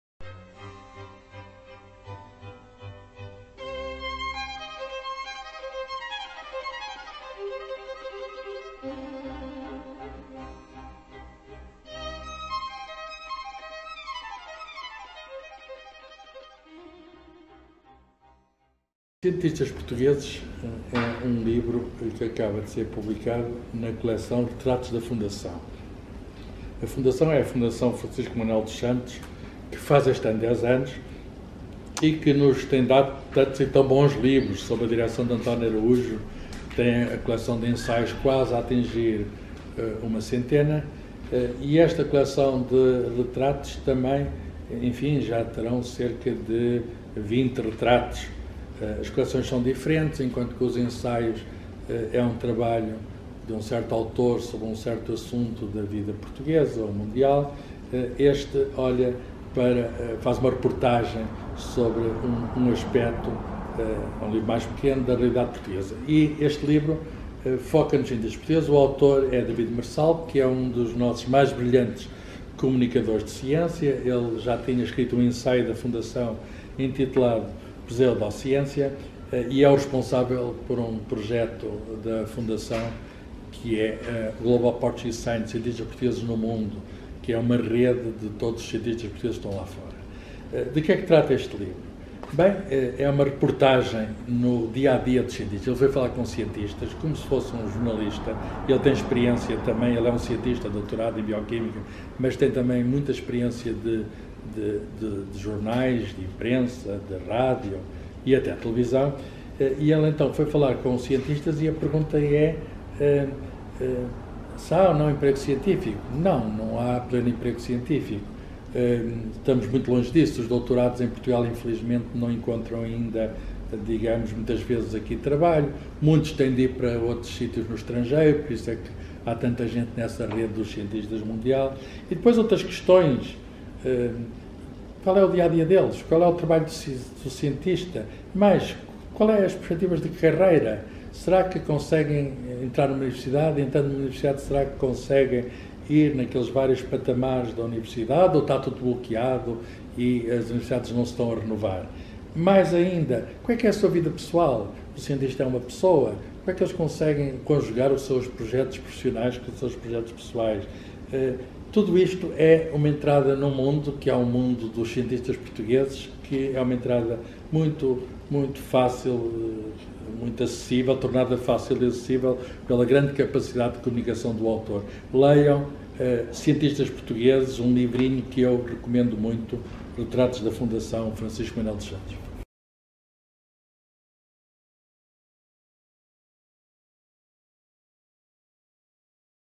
Ciência no Sofá (Ep. 47) "Cientistas portugueses" de David Marçal
No 47º episódio de "Ciência no sofá”, o espaço de divulgação de livros de divulgação científica do RÓMULO - Centro de Ciência Viva da Universidade de Coimbra, o Professor Carlos Fiolhais, sugere-nos a leitura da obra "Cientistas portugueses", da autoria de David Marçal. Publicado em Fevereiro de 2019, pela Fundação Francisco Manuel dos Santos.